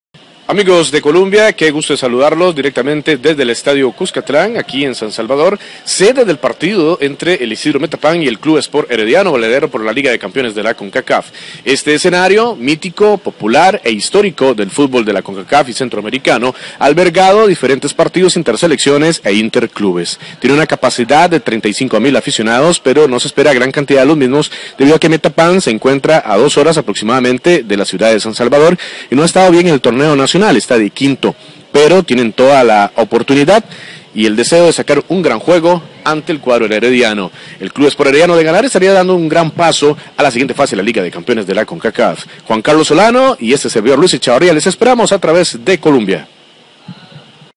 Información desde el Estadio Cuscatlán
En este escenario el Club Sport Herediano enfrentará al Isidro Metapán a las 8 pm